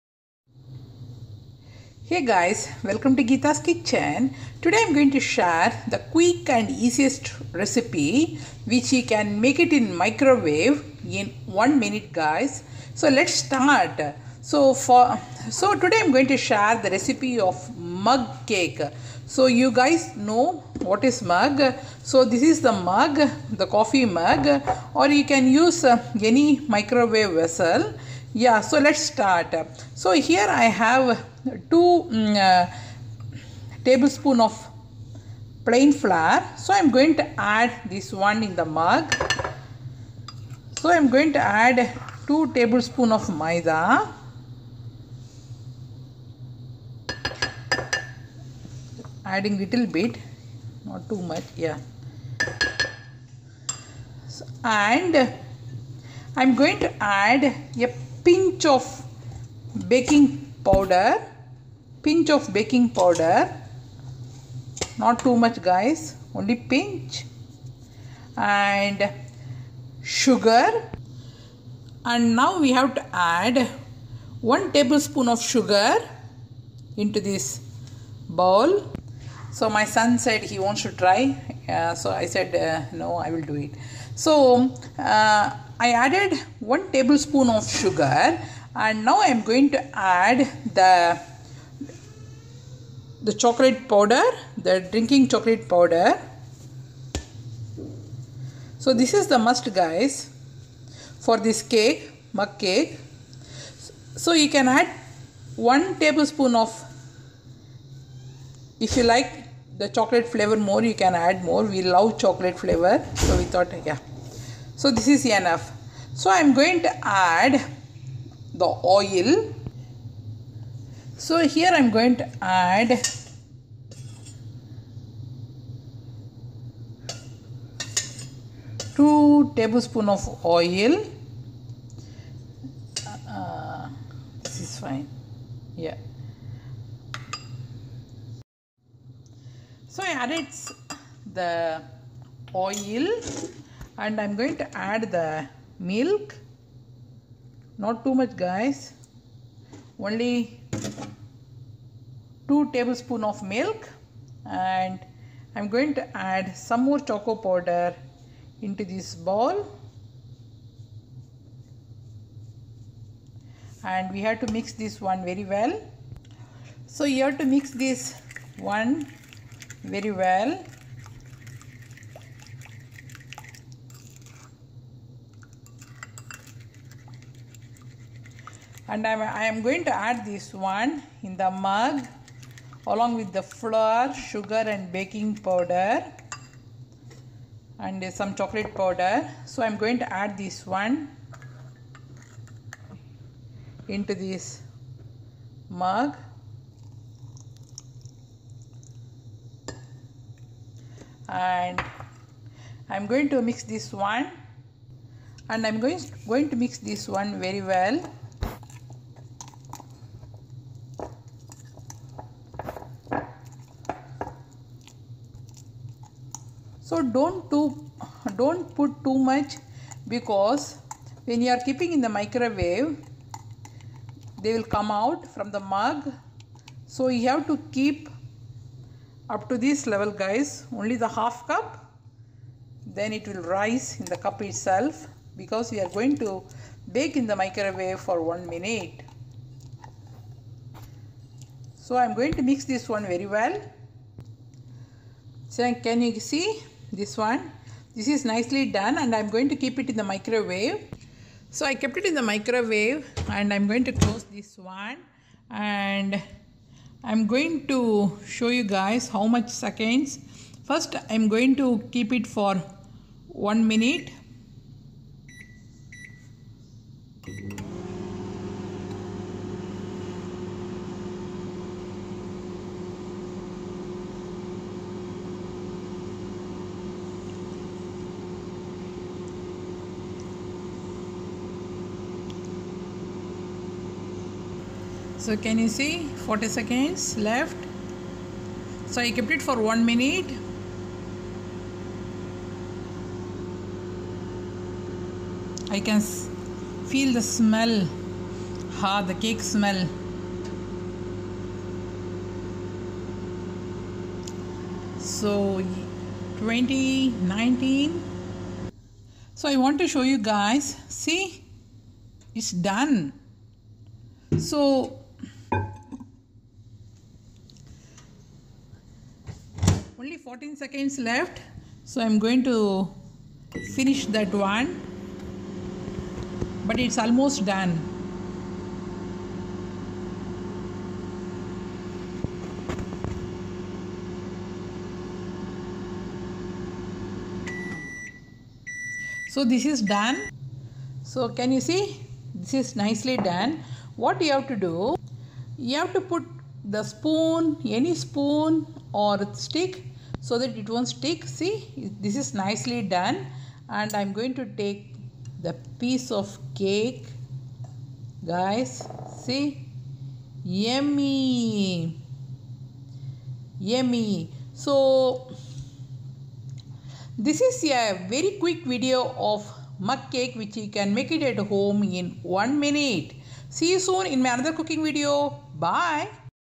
Mug cake in Microwave in 1 minute!
Hey  guys  today  i am  going  to  share  the  receipe  of  Mug  cake  which  u can make  at home  in 1 minute.  Please  try  this  recipe  at your  home.  You  can  have  this  Mug  cake  with  any  ice  cream  in the  top. Happy  cooking .